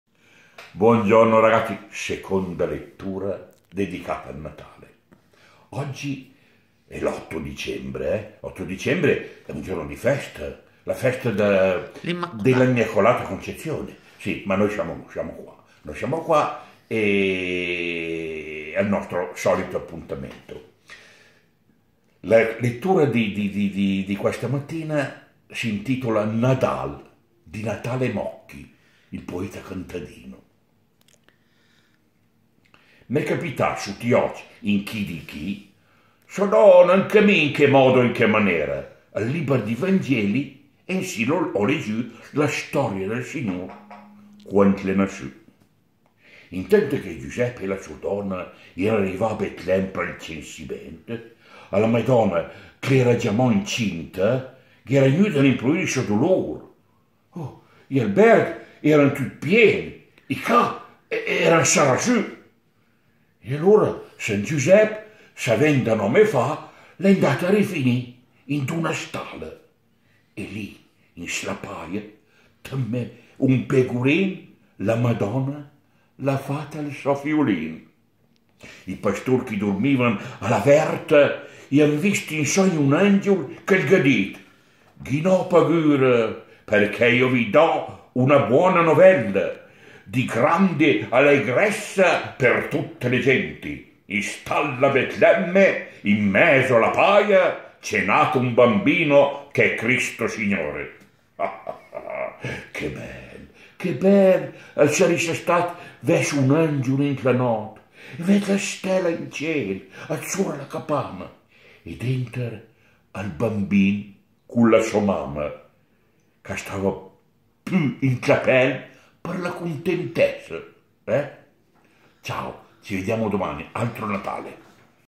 G.Manenti legge "Nadal" di Natale Mocchi
Video da parte di Walter Vai